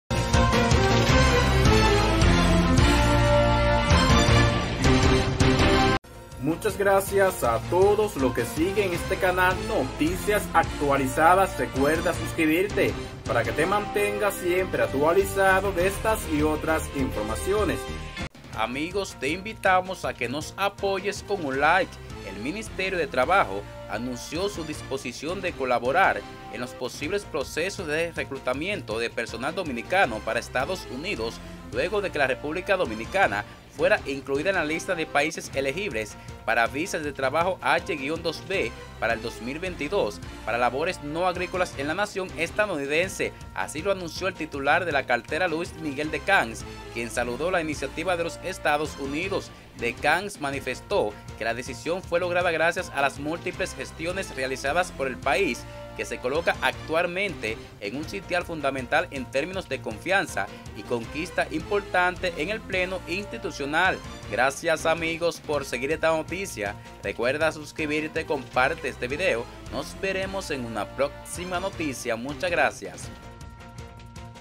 Ministerio de Trabajo se ofrece para reclutamiento de dominicanos que apliquen a visa H-2B de EE.UU
El Ministerio de Trabajo anunció este martes su disposición de colaborar en “los posibles procesos de reclutamiento” de personal dominicano para Estados Unidos, luego de que la República Dominicana fuera incluida en la lista de países elegibles para visas de trabajo H-2B para el 2022 para labores no agrícolas en la nación estadounidense.

Así lo anunció el titular de la cartera, Luis Miguel De Camps, quien saludó la iniciativa de los Estados Unidos.

De Camps manifestó que la decisión fue lograda “gracias a las múltiples gestiones realizadas por el país, que se coloca actualmente en un sitial fundamental en términos de confianza y conquistas importantes en el plano institucional”.